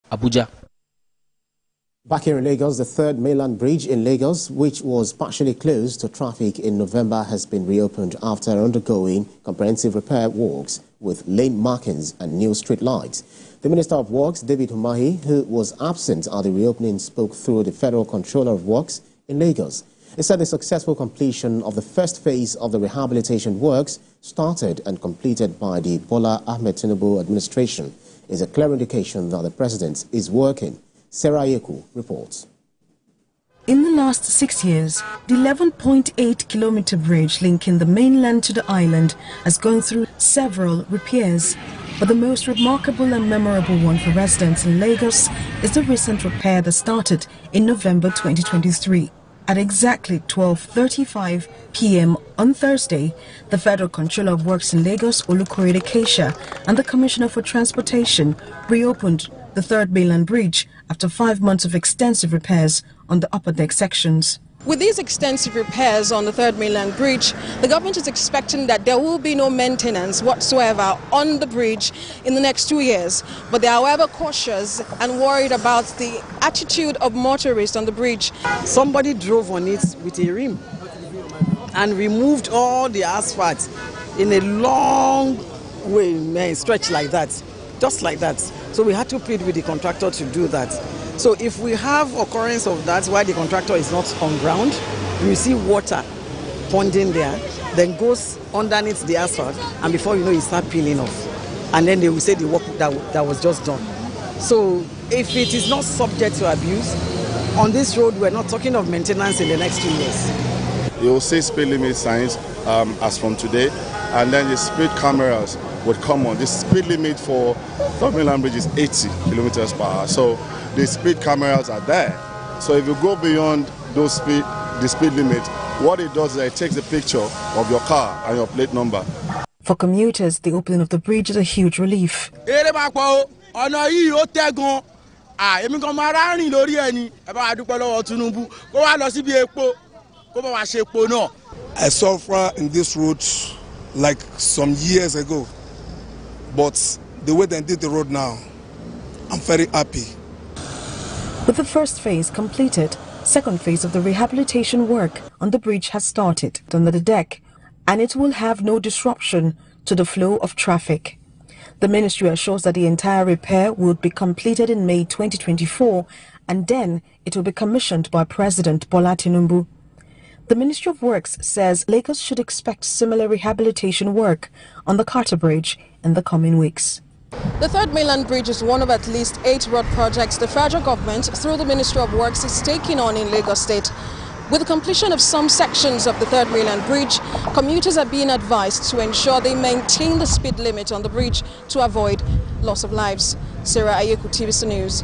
FG Opens Third Mainland Bridge After 5-Months Comprehensive Repair
The Third Mainland Bridge in Lagos, which was partially closed to traffic in November, has been re-opened after undergoing comprehensive repair works with lane markings and new streetlights.
The Minister of works, David Umahi who was absent at the reopening spoke through the federal controller of works in Lagos.
He said the successful completion of the first phase of the Rehabilitation works, started and completed by the Bola Tinubu administration is a clear Indication that the President is working.